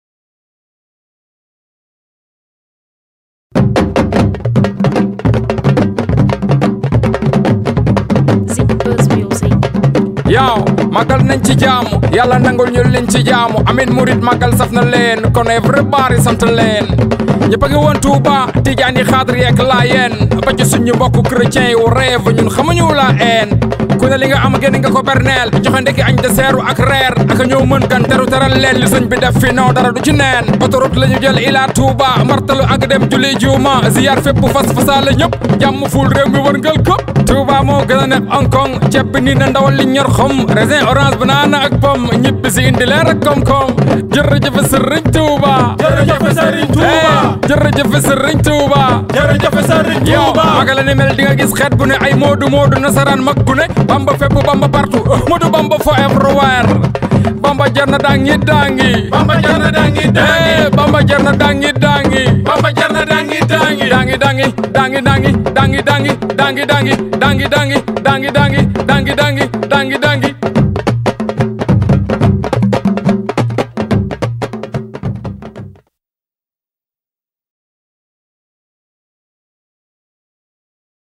Boutikou Laye - Actu Dose - Ep.67 (Retour Touba)
ça fut un plaisir de partager avec vous ce 18 SAFAR (2019). J'espère que vous avez adorer notre participation. Que le bon DIEU exauce tout nos prières.
N’hésitez pas à mettre vos suggestions en commentaire.
SERVICE COMMERCIAL "BOUTIKOU LAYE"  : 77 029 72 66 / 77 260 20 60

DIEUREU DJEUFEUTI SEURIGNE TOUBA!